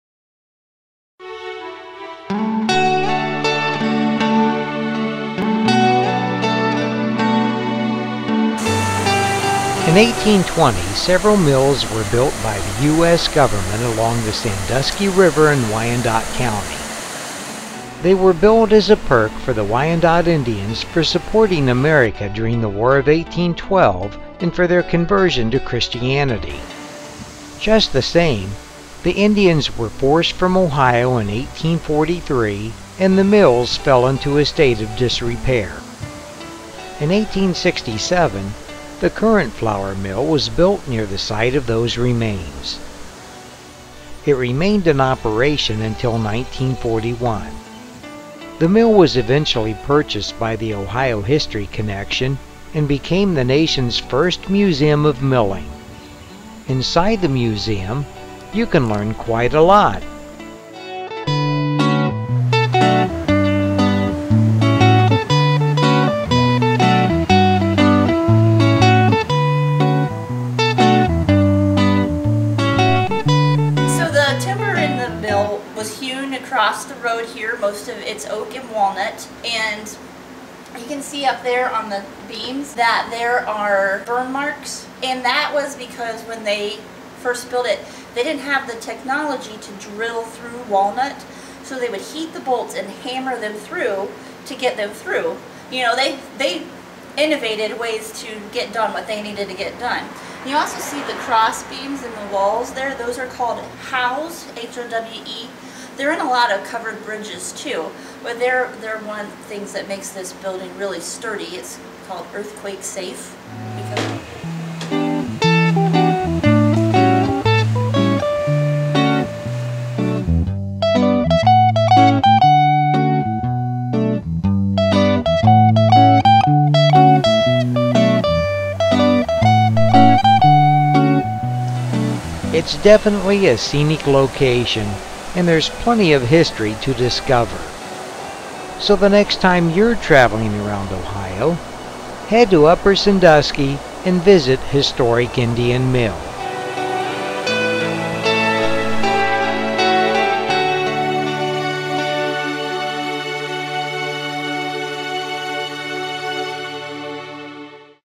Indian Mill (Upper Sandusky)
The original mills were built by the U.S. Government as a perk for the Wyandot Indians. The mill standing today was built in 1867 and now serves as the nation's first museum of milling. Located along the Sandusky River in Wyandot County it's a scenic trip worth taking. (Video recorded August 2014 and July 2015)